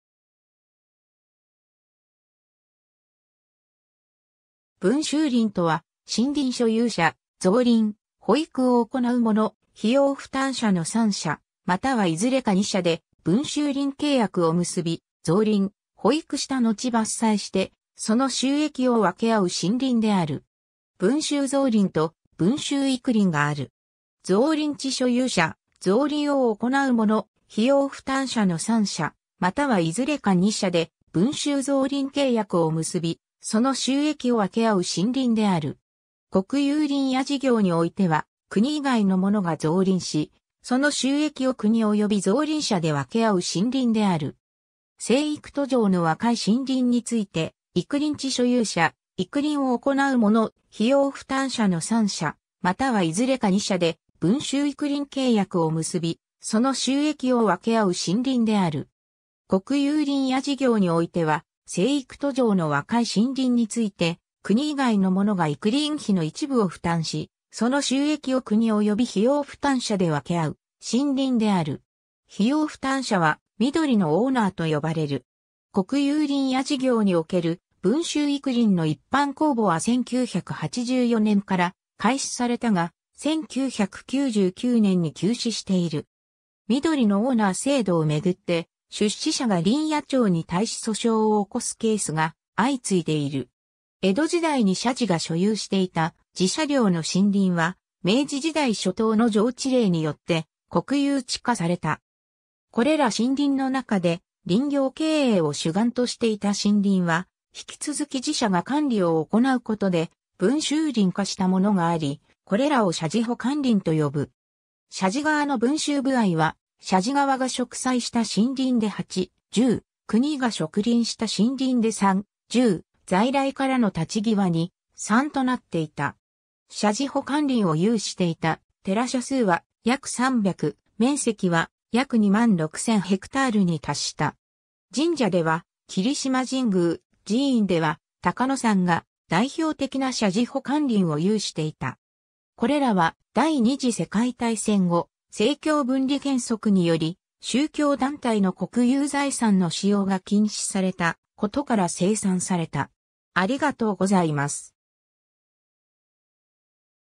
分収林
分収林（ぶんしゅうりん）とは、森林所有者、造林・保育を行う者、費用負担者の3者またはいずれか2者で分収林契約を結び、造林・保育したのち伐採して、その収益を分け合う森林である。
分収造林と分収育林がある。
 造林地所有者、造林を行う者、費用負担者の3者またはいずれか2者で分収造林契約を結び、その収益を分け合う森林である。
 国有林野事業においては、国以外の者が造林し、その収益を国及び造林者で分け合う森林である。
 生育途上の若い森林について、育林地所有者、育林を行う者、費用負担者の3者またはいずれか2者で分収育林契約を結び、その収益を分け合う森林である。
 国有林野事業においては、生育途上の若い森林について国以外の者が育林費の一部を負担し、その収益を国及び費用負担者で分け合う森林である。
費用負担者は「緑のオーナー」と呼ばれる。
国有林野事業における分収育林の一般公募は1984年から開始されたが、1999年に休止している。
 『緑のオーナー』制度を巡って、出資者が林野庁に対し訴訟を起こすケースが相次いでいる。
 江戸時代に社寺が所有していた寺社領の森林は、明治時代初頭の上知令によって国有地化された。
これら森林の中で林業経営を主眼としていた森林は、引き続き寺社が管理を行うことで分収林化したものがあり、これらを社寺保管林（しゃじほかんりん）と呼ぶ。
社寺側の分収歩合は、社寺側が植栽した森林で8/10、国が植林した森林で3/10、在来からの立ち木は2/3となっていた。
社寺保管林を有していた寺社数は約300、面積は約2万6000haに達した。
神社では霧島神宮、寺院では高野山が代表的な社寺保管林を有していた。
これらは第二次世界大戦後、政教分離原則により宗教団体の国有財産の使用が禁止されたことから清算された。